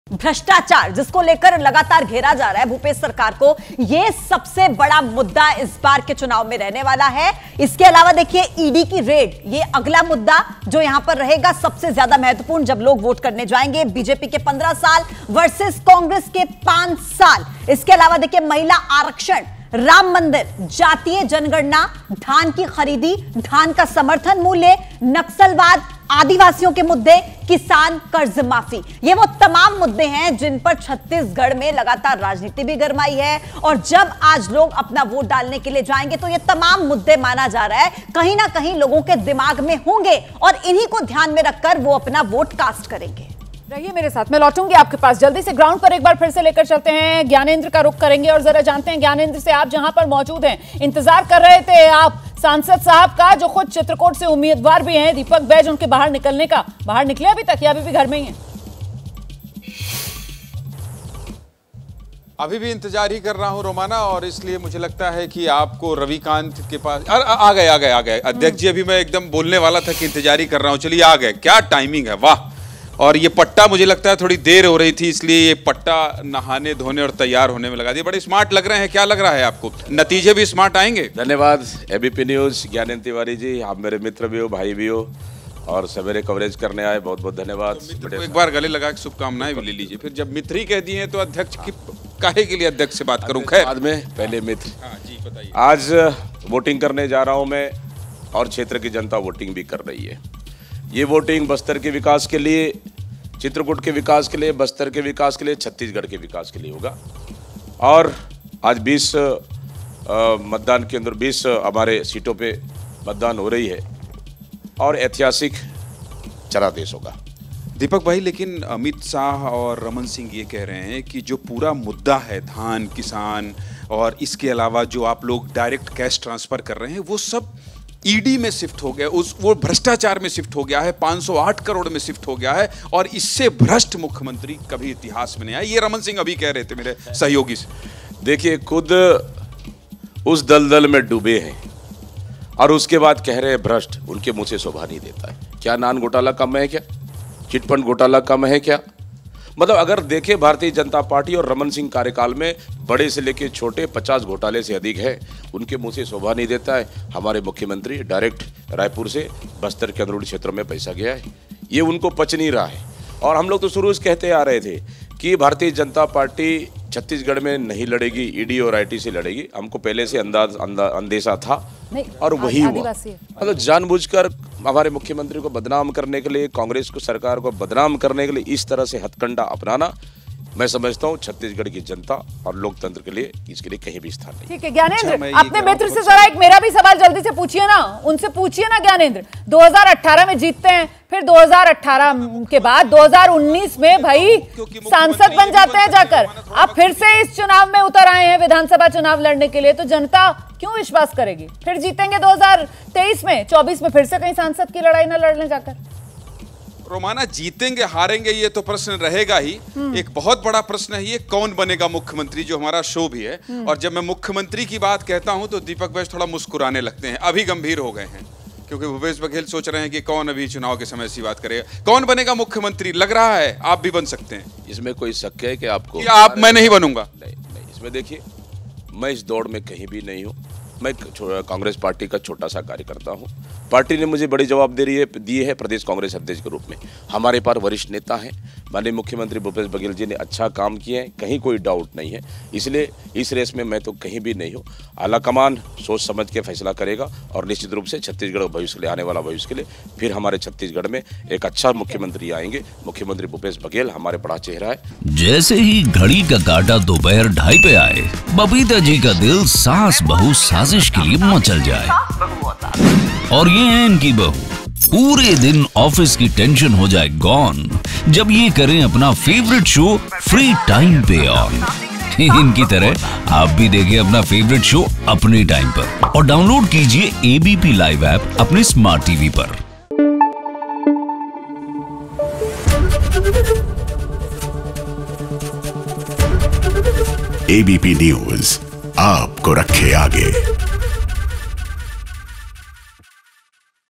CG Election Voting : जगदलपुर में वोट देने से पहले क्या बोले कांग्रेस प्रत्याशी ? | Congress
छत्तीसगढ़ विधानसभा चुनाव के लिए पहले चरण की 20 सीटों पर वोटिंग शुरू हो गई है. मिजोरम में एक ही चरण में 40 सीटों पर वोटिंग की शुरुआत भी हो गई है. लोगों ने सुबह 7 बजे से वोट डालना शुरू किया.